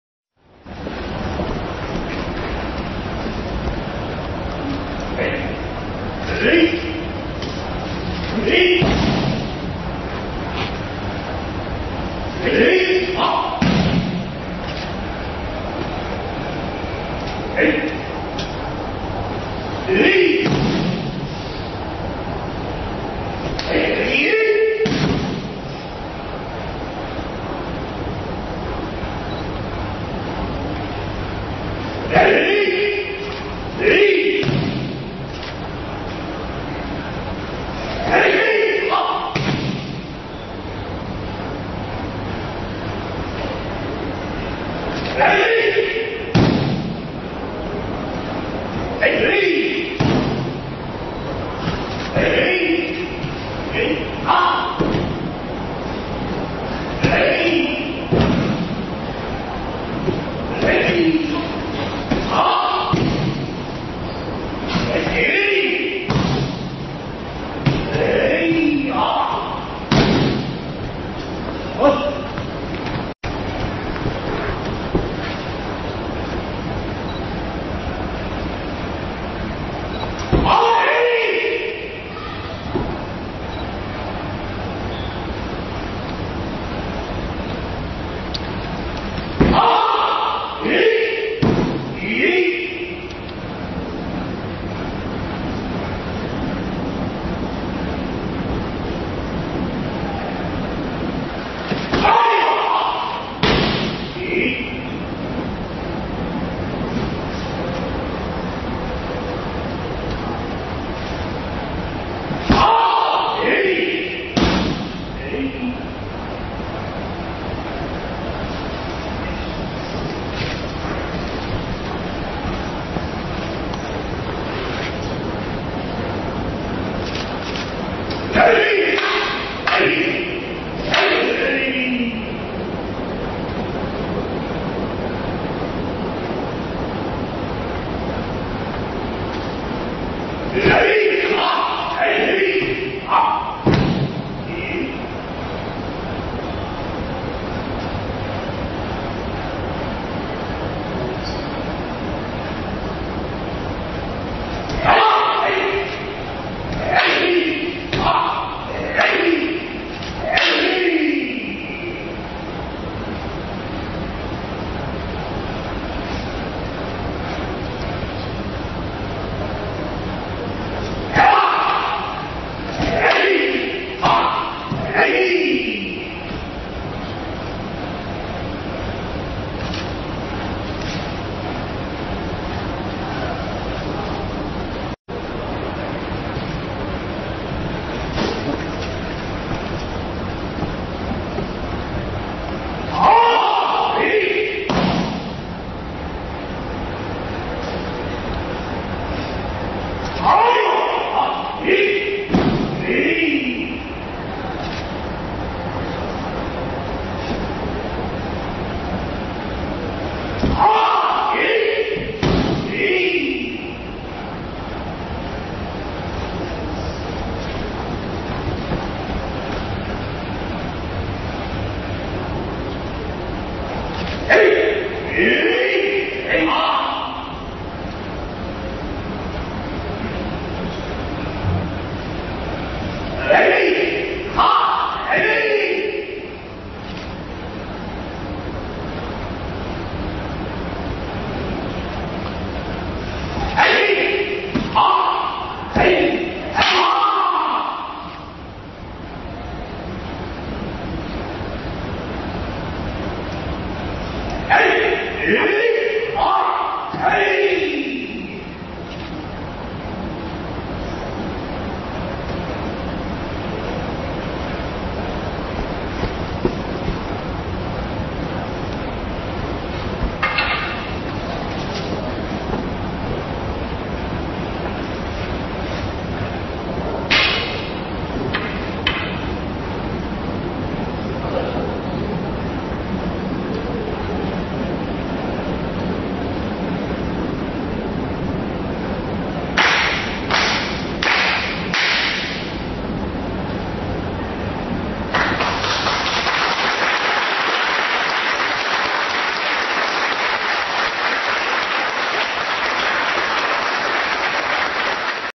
Aikido Iwama Ryu
Ивама Синсин Айки Сюренкай, Показательная демонстрация айкидо ивама.
Хитохиро Сайто